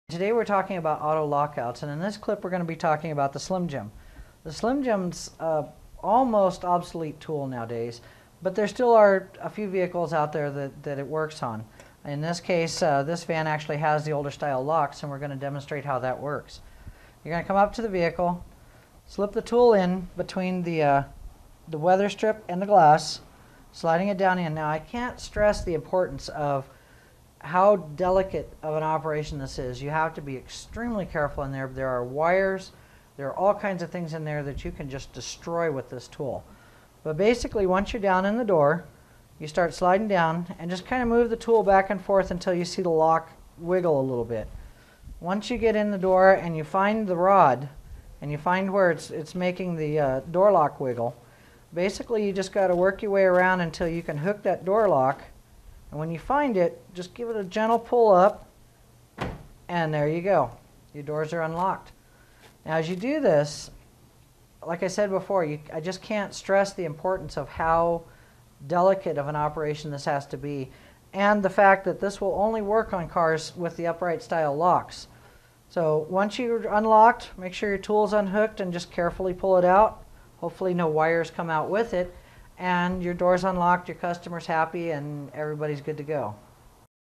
Slim Jim Instructions
Slim Jim Instructions. Part of the series: How to Retrieve Keys Locked in a Car. Use a slim jim tool for car lockouts without damaging the inside of the door; learn how from our expert locksmith in this free auto emergency locksmith-training video.